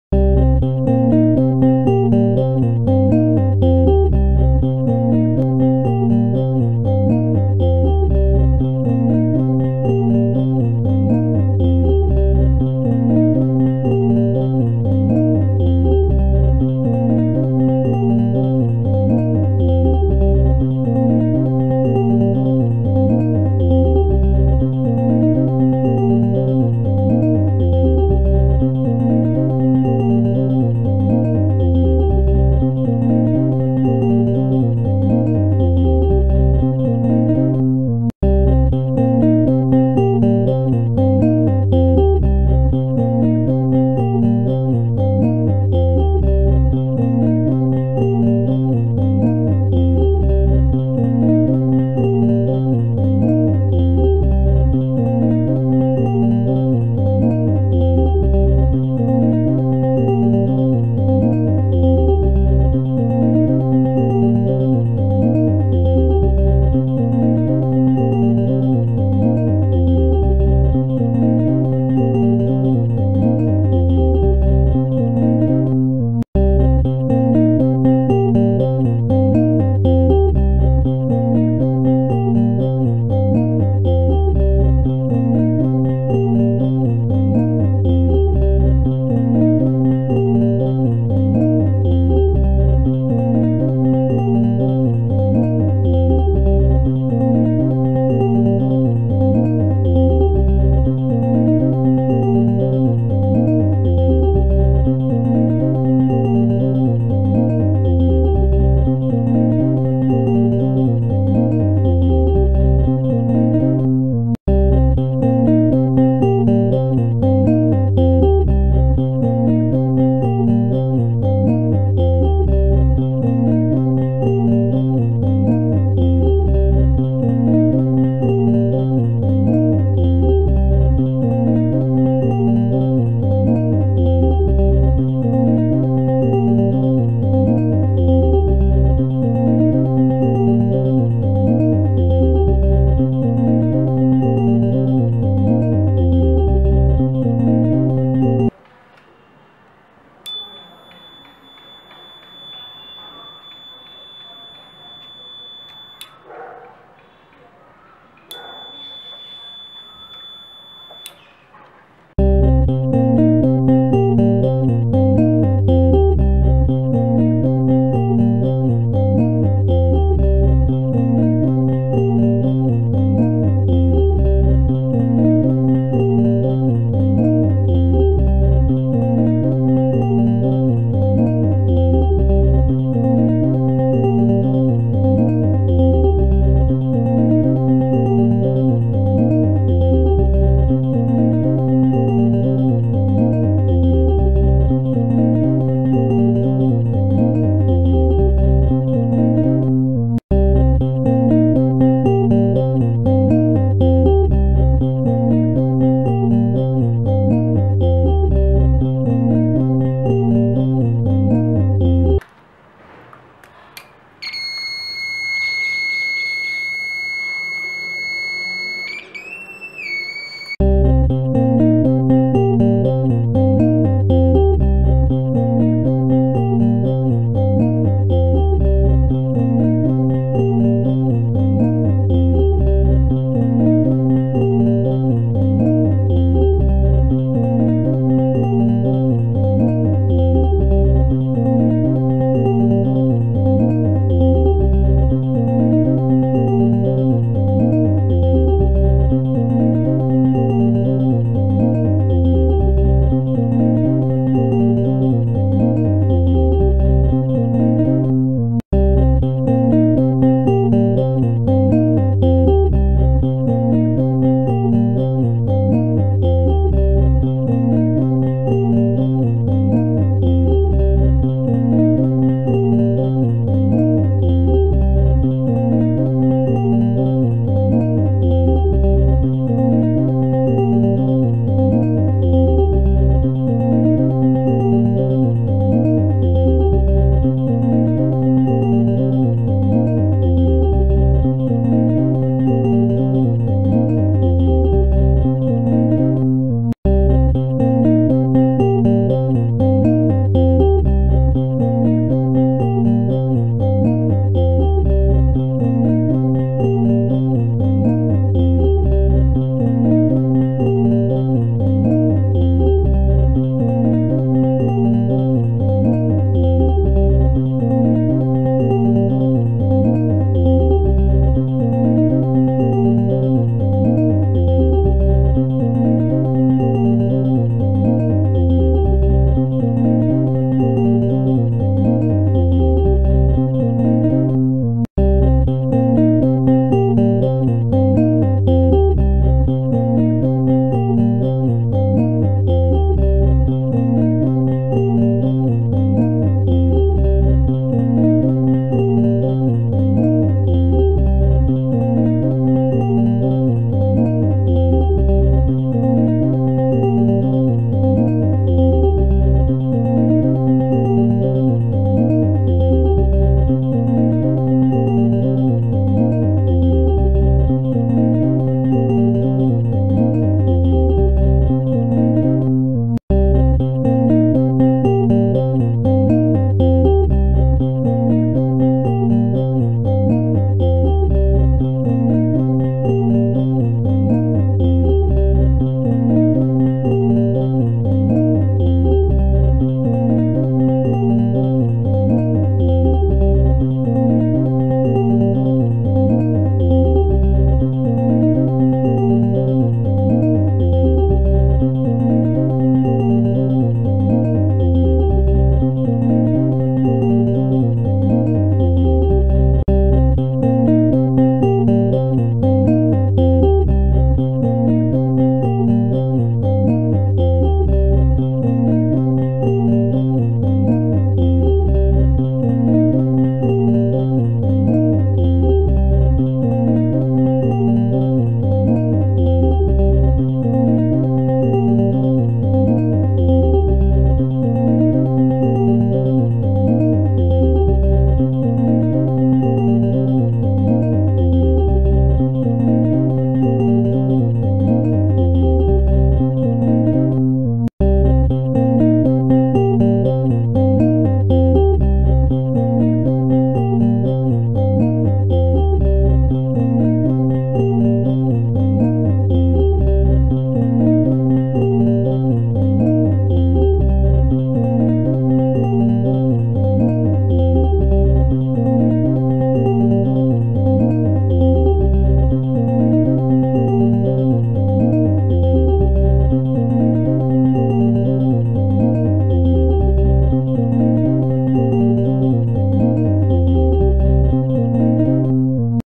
One thing, multiple uses : ONE Concept, 3 COOL Things | Build Fan, Produce Sound and Light
A simple circuit with different loads like  buzzer, DC toy motor and LED. This circuit explains the concept of different types of loads.
Subtitles in English, Hindi, Kannada and Marathi